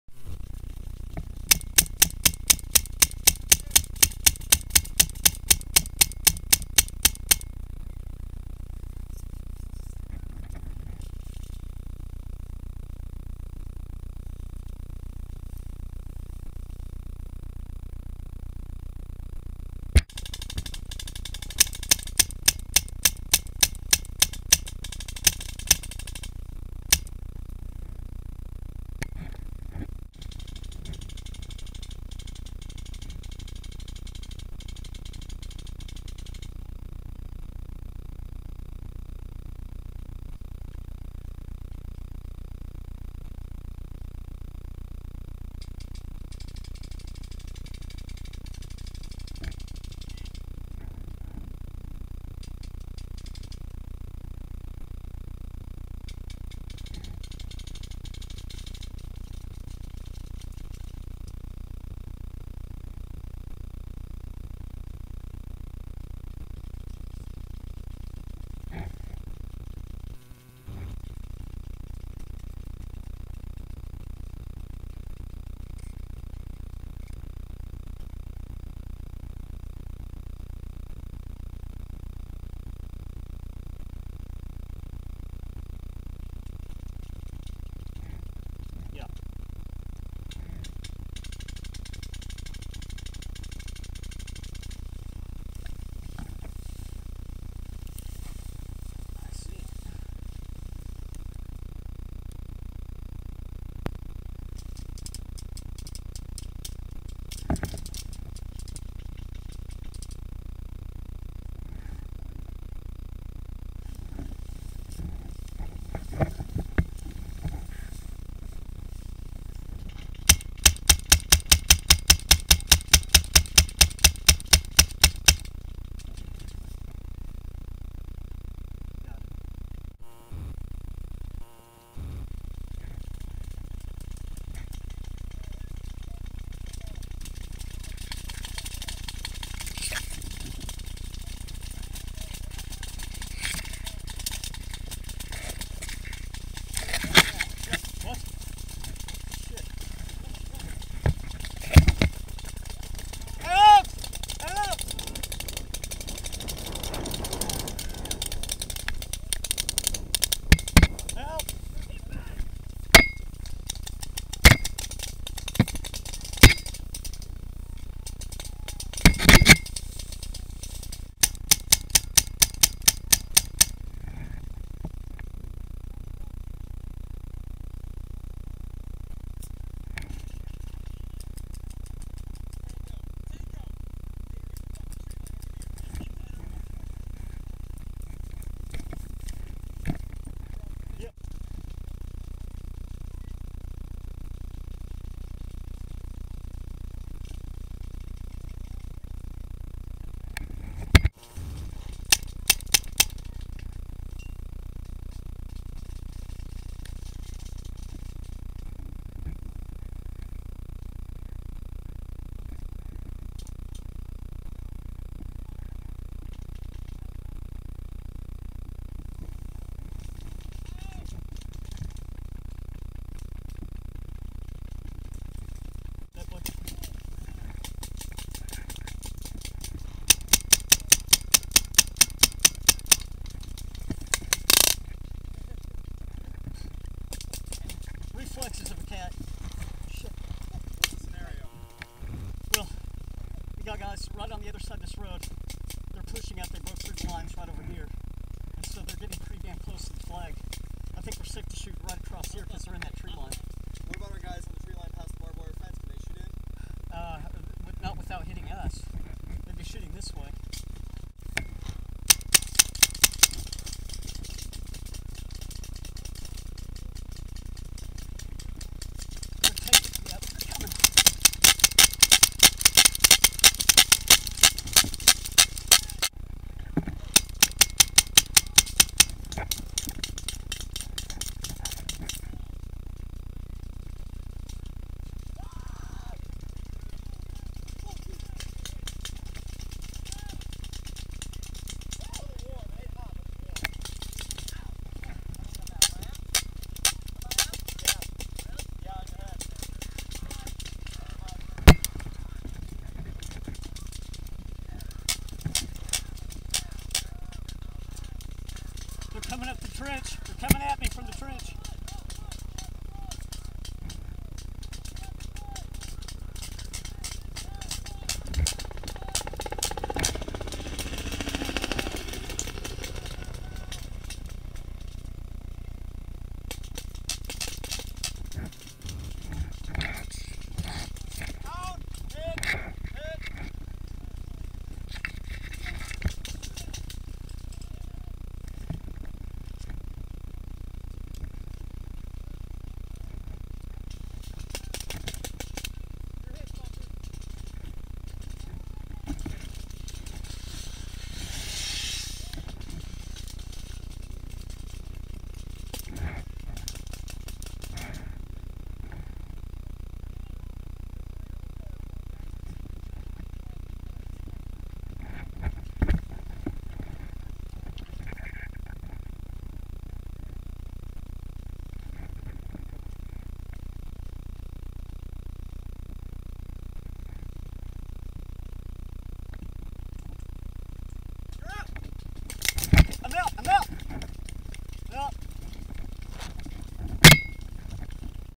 Brecourt Manor -- Oklahoma D-Day 2010
Some helmet cam clips of the action at Brecourt Manor at Oklahoma D-Day 2010.  Shot by, "SicEm", of the 914th GIR.